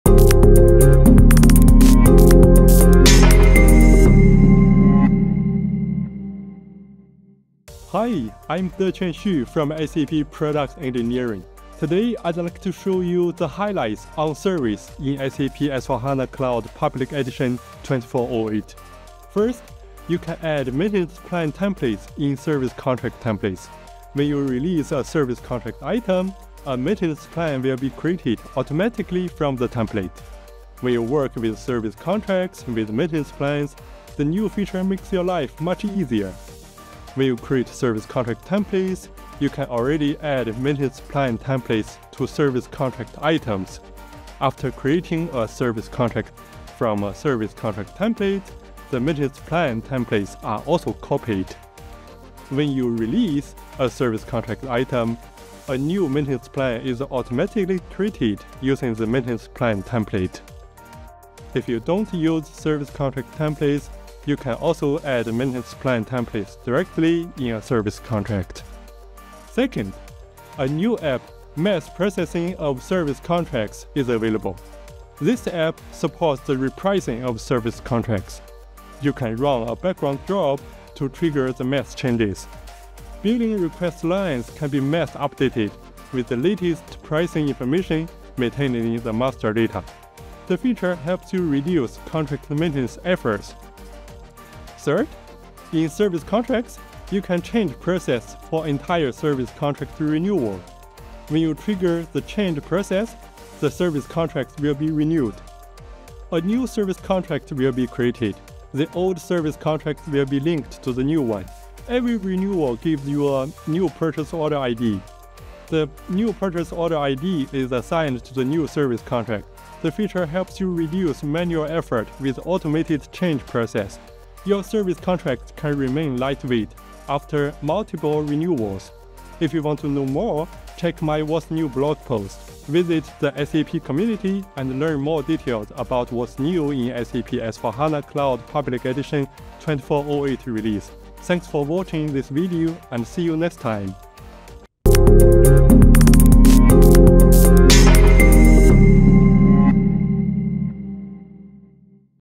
Service in SAP S/4HANA Cloud Public Edition 2408
🔔Dive into the latest service features in SAP S/4HANA Cloud Public Edition 2408. This video showcases the new tools and enhancements aimed at improving service management. Join us to discover how these updates can optimize your service operations and benefit your business.

The key highlights shared in this video are:
00:19 Ability to Add Maintenance Plan Templates in Service Contracts
01:09 Application for Mass Updates of Service Contracts
01:34 Change Process for Entire Service Contract Renewal